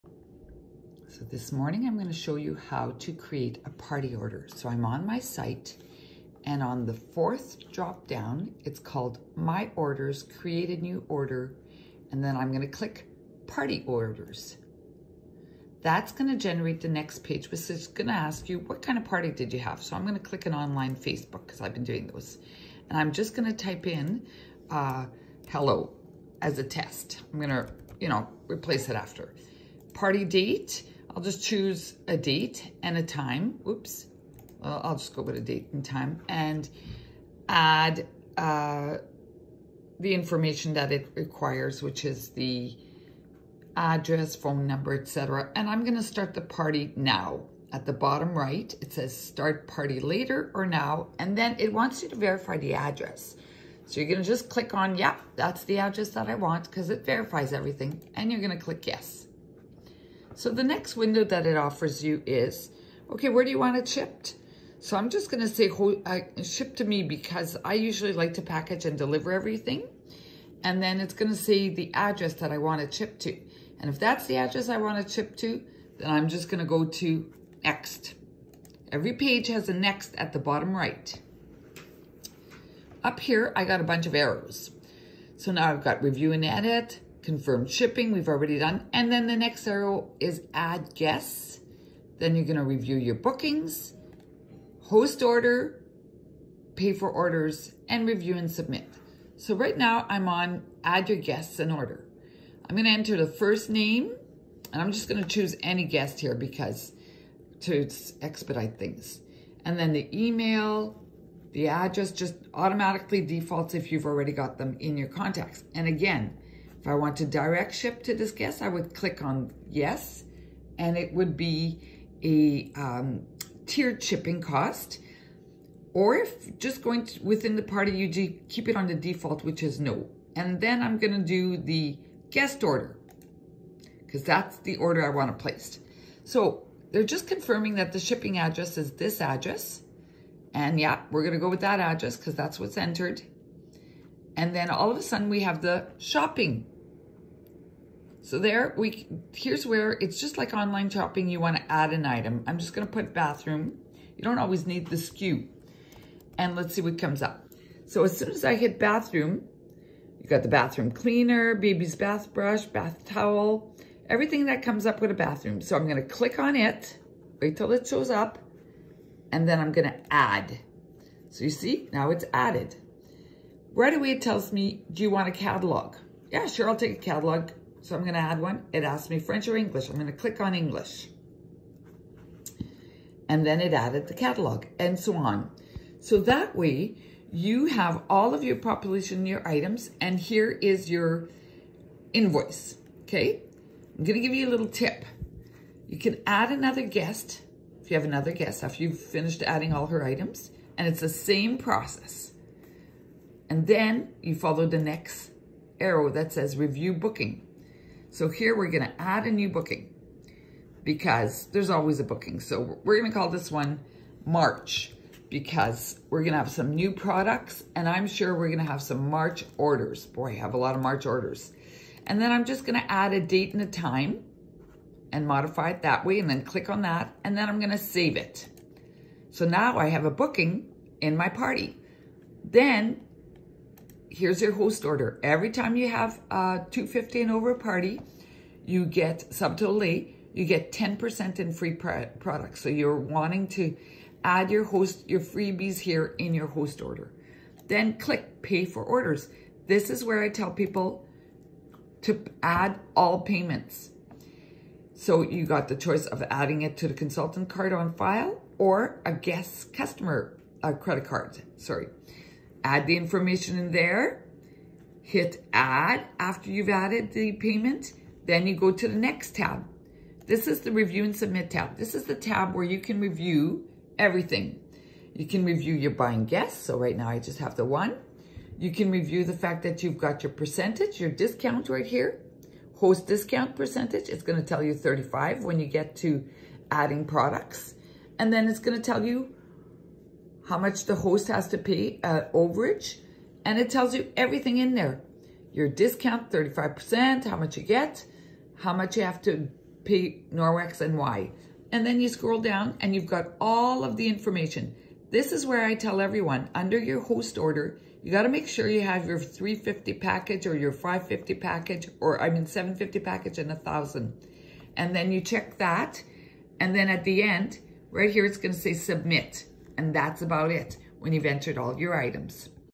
How to create a Party on your site
Step by step on how it create a party from your site.